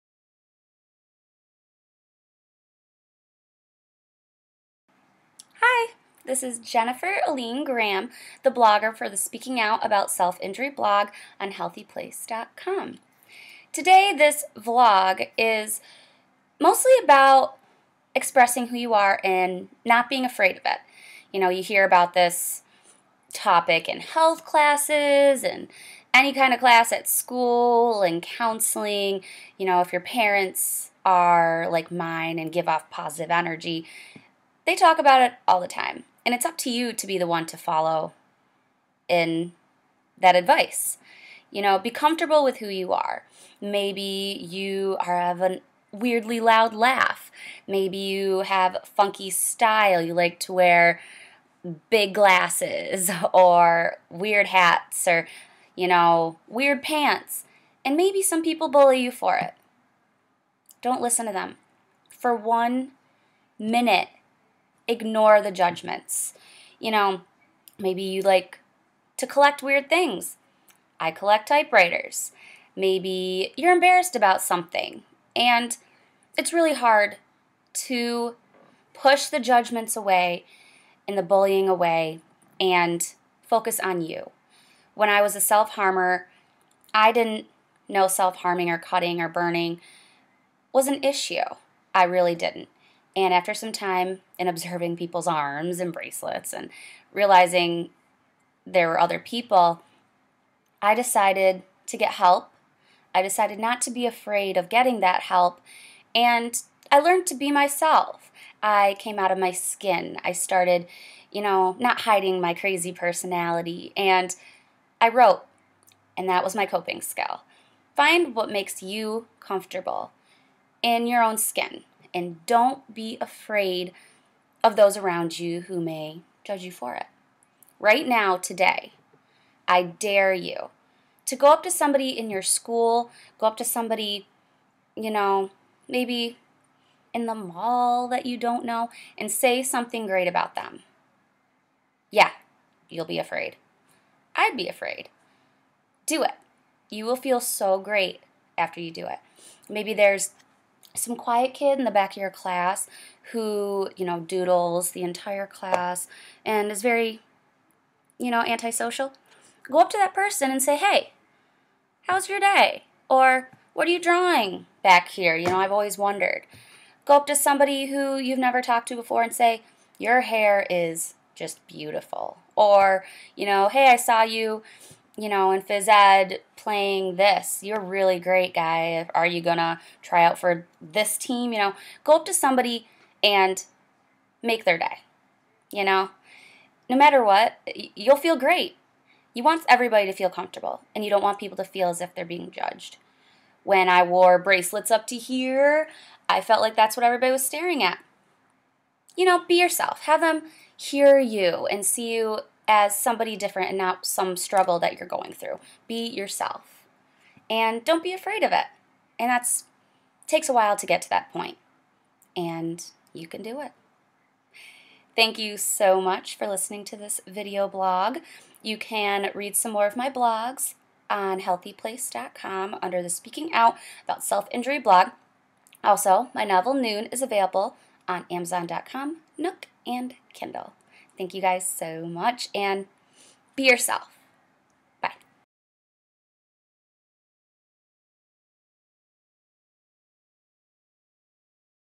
Be Comfortable in the Skin You're In - Scarred or Not
Self-harm scars can make it tough to be comfortable in the skin you're in. So what can you do to build your self-esteem? I've got a small challenge for you. Watch now.
Self-Harm and Being Comfortable in the Skin You’re In: A Video Blog